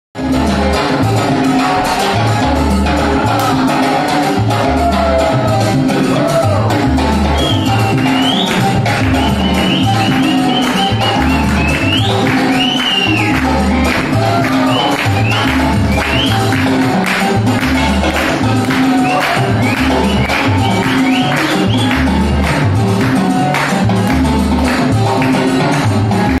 Richie Torres showing off his stuff!
At Kingslander, Auckland. Richie Torres and his awesome musicality and shines